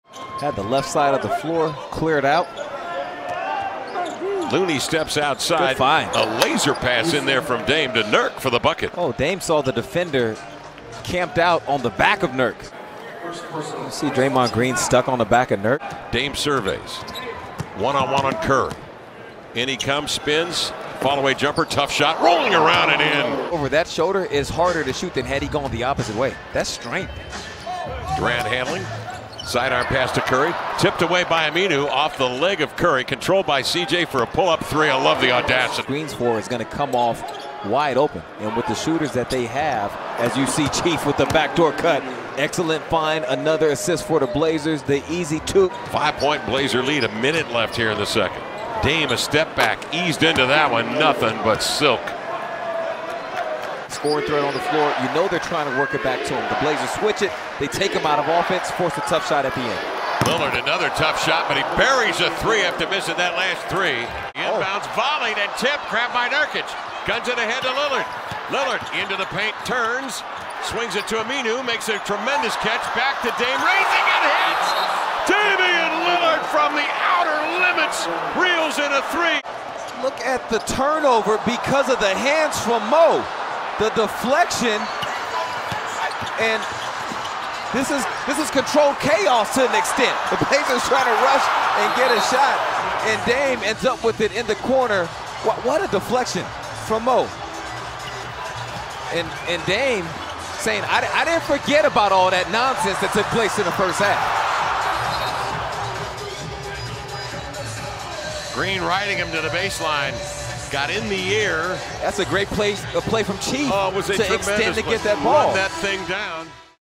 Damian Lillard (21 points) Highlights vs. Golden State Warriors | December 27, 2018
27 points, 5 assists, and 1 game-winner for the kid from Oakland.